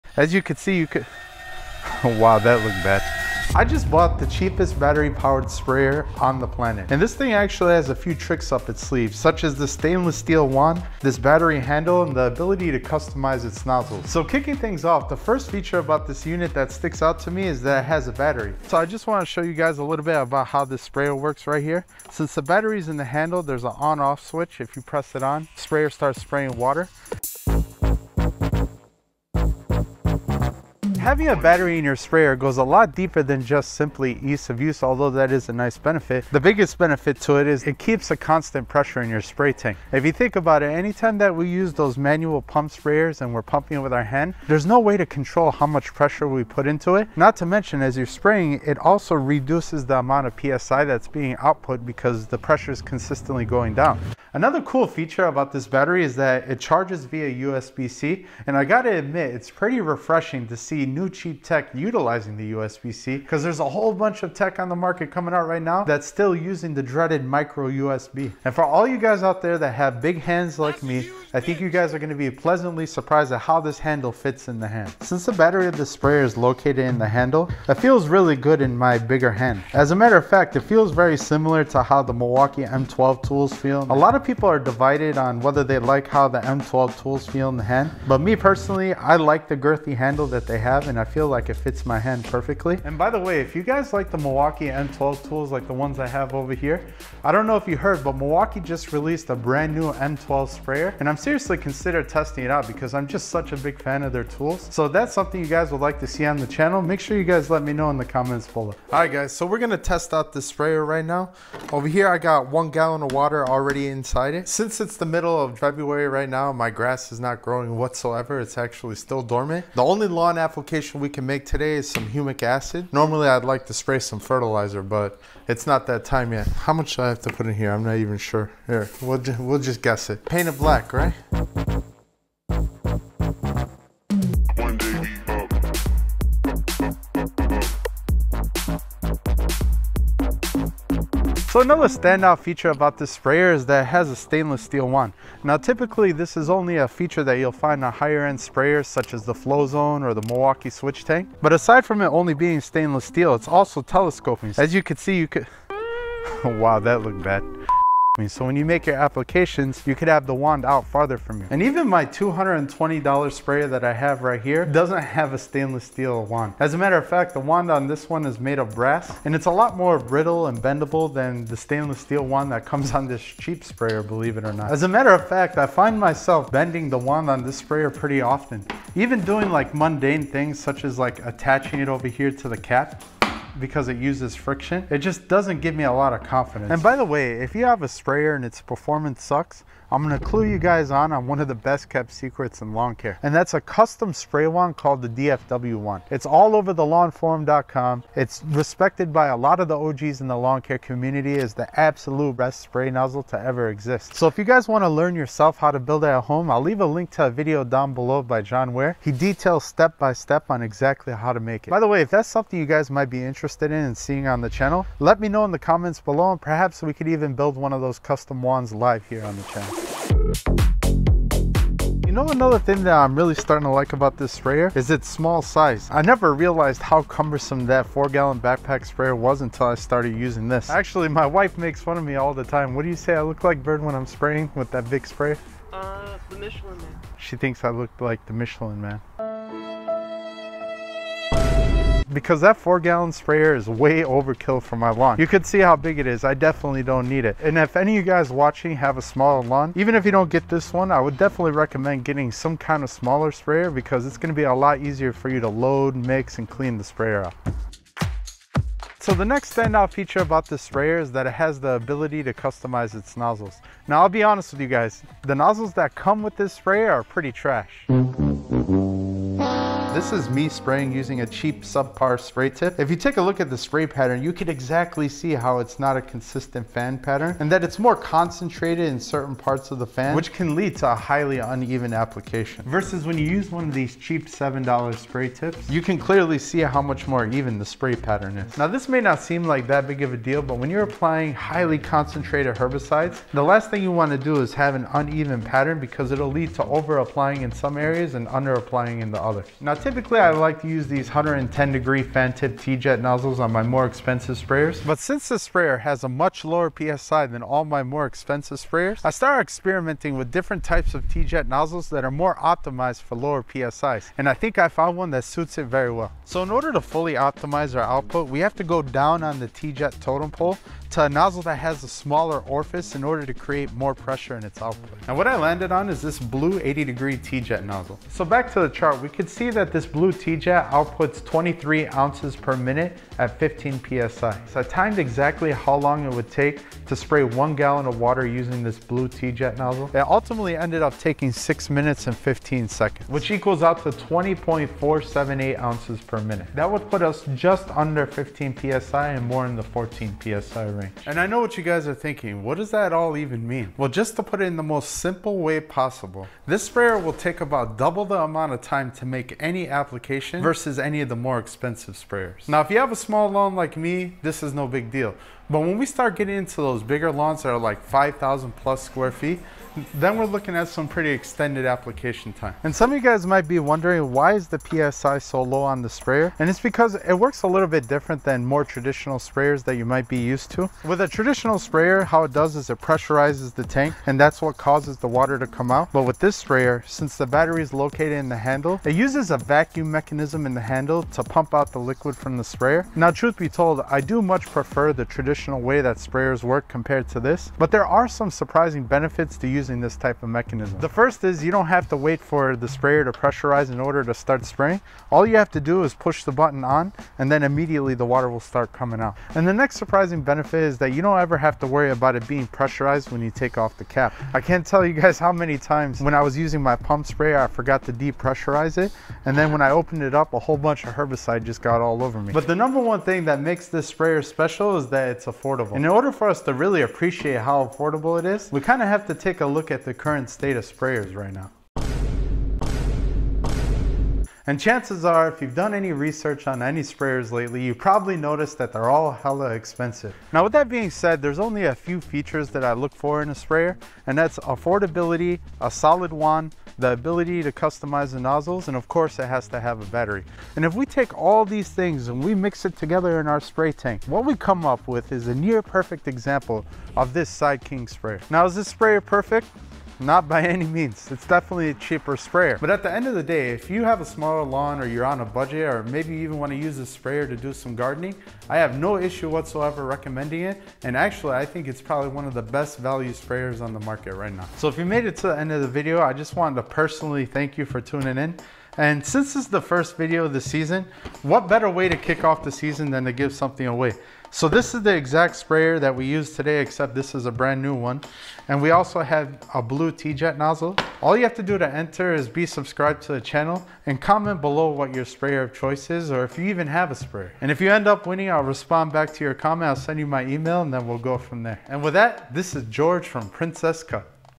I Tested the World’s CHEAPEST Battery Sprayer
And To My Surprise… It’s Actually Not That Bad!

In this Video, we will show you the world’s Cheapest Battery Sprayer. The SideKing Battery Sprayer is currently on sale on Amazon for $38.00. It features a 1.35 gallon spray tank, battery powered handle, and adjustable spray nozzles.

This sprayer is battery powered which is preferred for more than just convenience, its greatest strength is its able to spray at a constant PSI which is extremely important when making highly precise applications such as the commercial grade herbicides like the ones we use on the channel.

This sprayer also has a stainless steel wand, you will typically find this feature on higher end sprayers such as the Dewalt backpack sprayer , Milwaukee m12 sprayer and the Echo backpack sprayer.

This sprayer also has the ability to customize its sprayer nozzles. The nozzles that come with sprayer are fine for general gardening or to use as a sprayer for plants but if you will be applying liquid lawn fertilizer or will be using it as a weed sprayer with herbicides, they will need to be upgraded. The best way to upgrade this sprayer is with a Teejet nozzle. After a bit of testing I came to the conclusion that the best nozzle to use with this sprayer is the blue Teejet XR8003VS. Once attached, it will spray an 80 degree fan at 14 PSI and will take 6 min and 15 seconds to spray 1 gallon of water. Keep in mind this is half the PSI of more expensive sprayers such as the sprayers plus 105ex I normally use.

Overall this sprayer is a much better budget option than buying a pump sprayer but for heavy duty lawn care jobs and larger lawns, you will be much better off buying a sprayer with a higher PSI and a larger tank capacity.

➡️ Items Used:

Items Needed To Upgrade Nozzle:
Teejet Nozzle